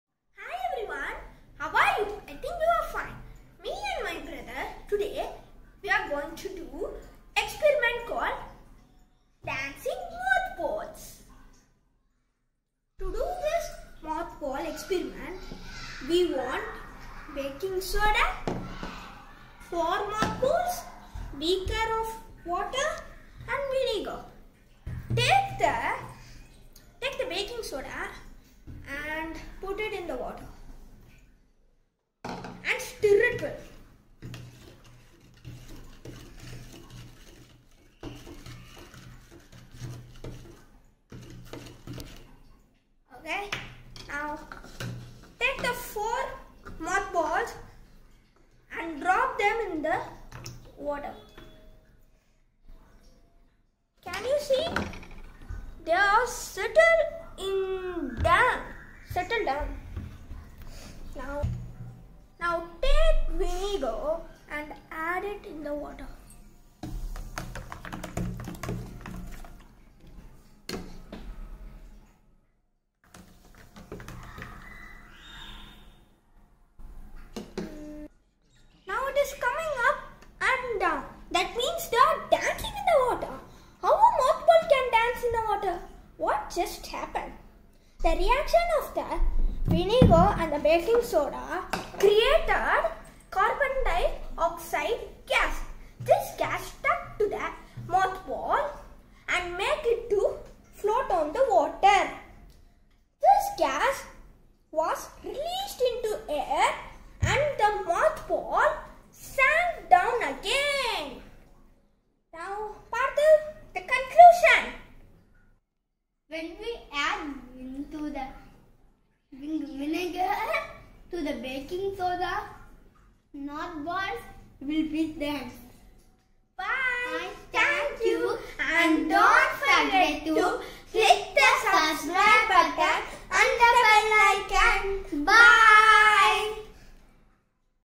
Dancing Mothballs...
how mothballs dance in water. concept explained...kindly watch till end..